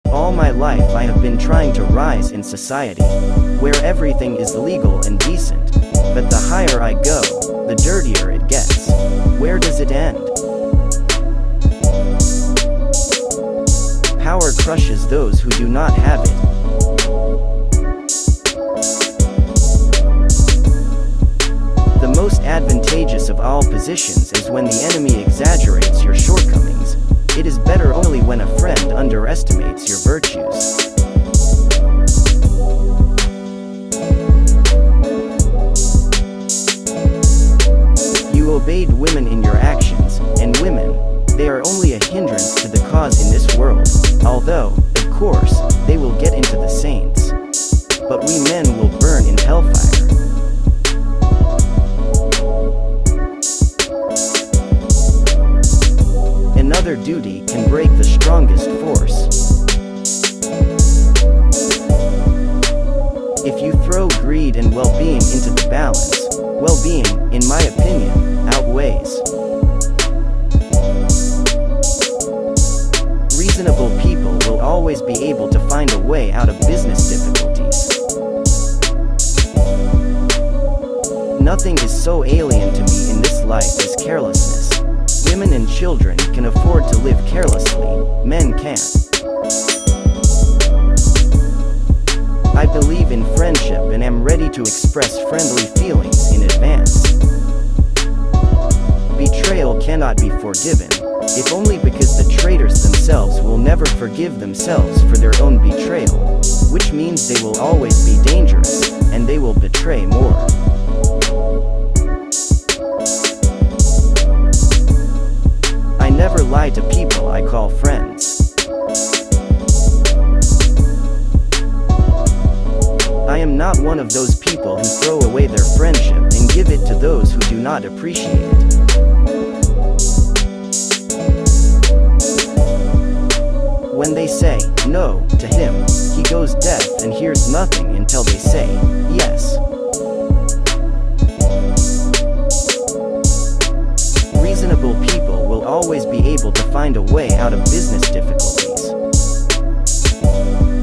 Frank Costello (the prototype of Vito Corleone).
An American gangster of Italian origin. Nicknamed the "Prime Minister of the Underworld," he was one of the most powerful and influential mafia bosses in the United States.